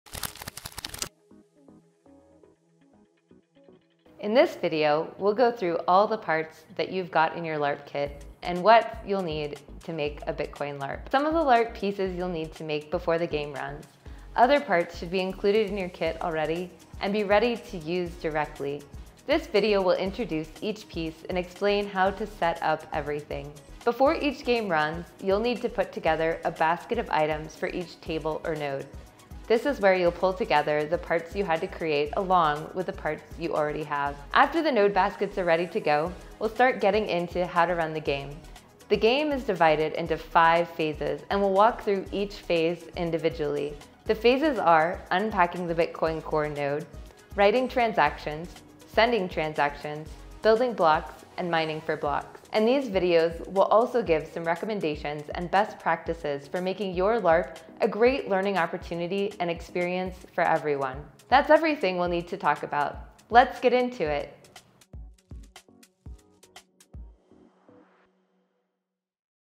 02 - Knowing the game
This is the second video for the LARP facilitators. This will guide you through how the game is played for you to be aware what's expected to happen.